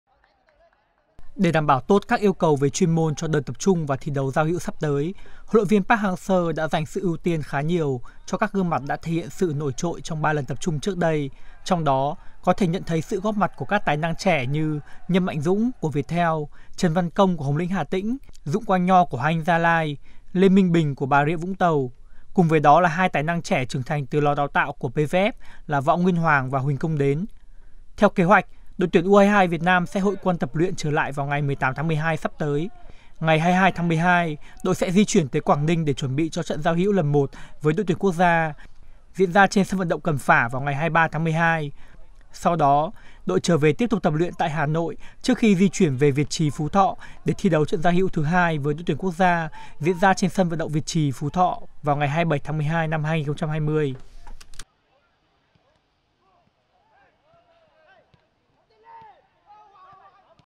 HLV Park Hang Seo công bố danh sách triệu tập ĐT U22 Việt Nam chuẩn bị cho 2 trận giao hữu với ĐTVN
U22 Việt Nam trong đợt tập trung này thiếu vắng 1 số gương mặt quan trọng